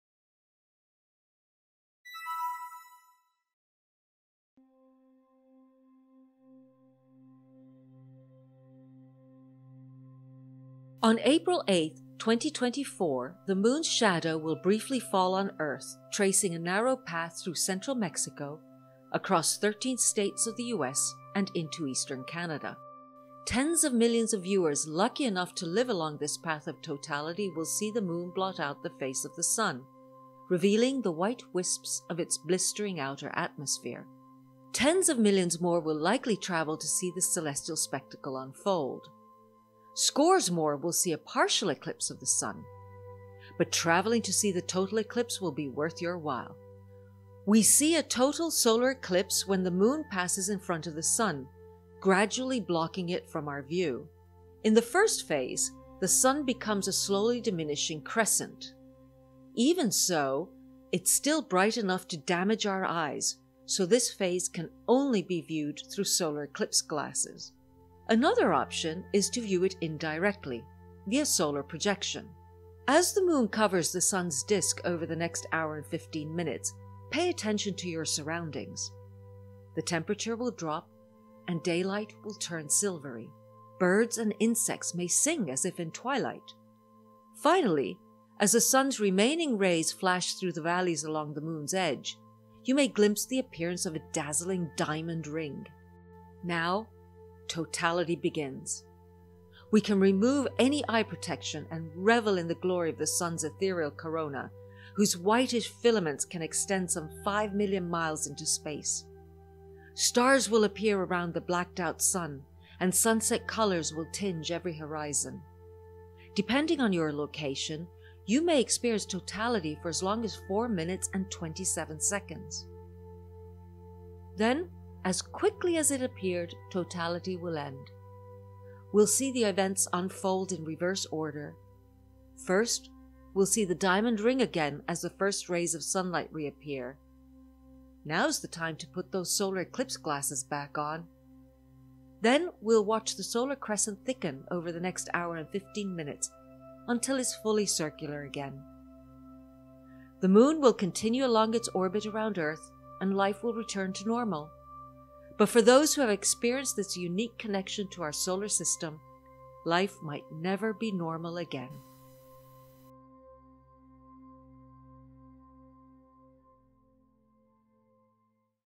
The 2024 Total Solar Eclipse | Viewing and Info | April 8, 2024
On April 8, 2024, North America will be in for a real treat: a total solareclipse . That’s when the Moon will blot out our nearest star for lucky viewers along a narrow strip of land that stretches from central Mexico, across Texas, up through NewEngland, and into the Canadian Maritimes. Observers across NorthAmerica will see a partial eclipse, provided skies are clear.

For more news about the solar eclipse and all the info you need about eclipse glasses, the path of totality, and more, visit skyandtelescope.org.